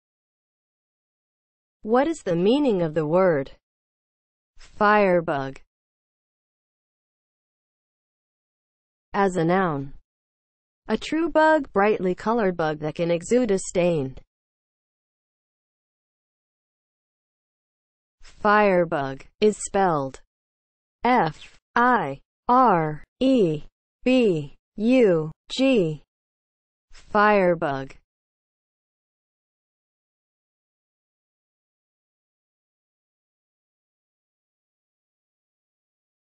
What is the meaning of the word FIREBUG?
Definition and spelling of the word FIREBUG.

(00:00) Intro
(00:08) As a noun
(00:17) Spelling

As a noun (00:08):
• a true bug: brightly colored bug that can exude a stain